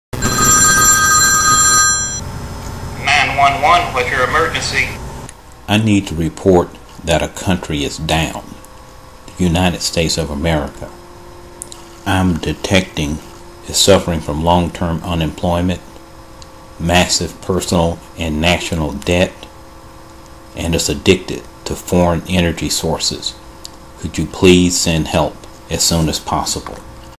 911 Country Down
Books by D T Pollard 911 -- Country Down is a look at what is ailing the United States of America and what actions need to be taken in order to get the nation back on the road to prosperity and growth.